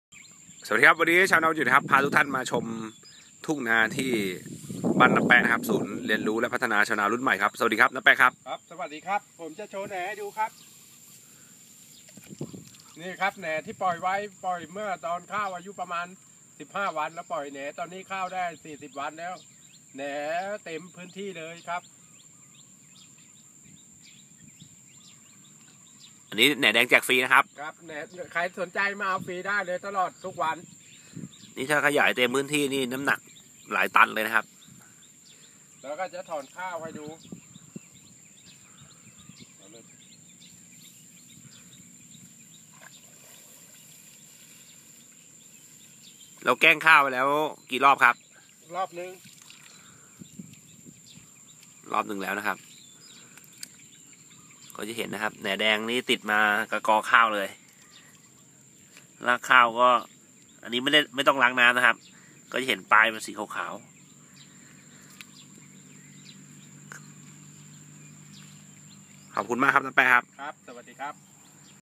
ขุมทรัพย์แหนเเดง นางลือ ชัยนาท
แหนเเดง แจกฟรี @ศูนย์เรียนรู้และพัฒนาชาวนารุ่นใหม่ ต.นางลือ อ.เมือง จ.ชัยนาท

ติดตามได้ที่ เพจ ชาวนาวันหยุด